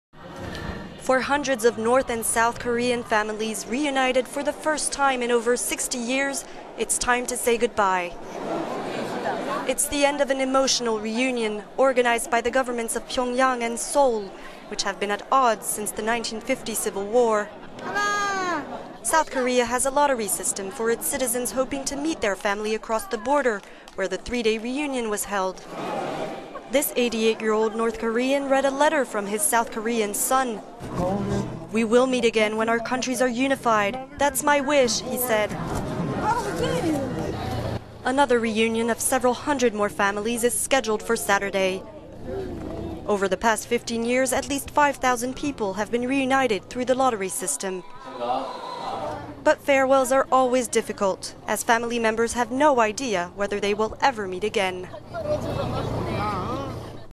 Tears as reuinited North & South Korean families bid emotional farewells
For hundreds of North and South Korean families "reunited for the first time in over 60 years, it was time on Thursday (October 22) to say goodbye.

The emotional farewells marked the end of a "three-day reunion" organised by the governments of Pyongyang and Seoul, which have been at odds since the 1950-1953…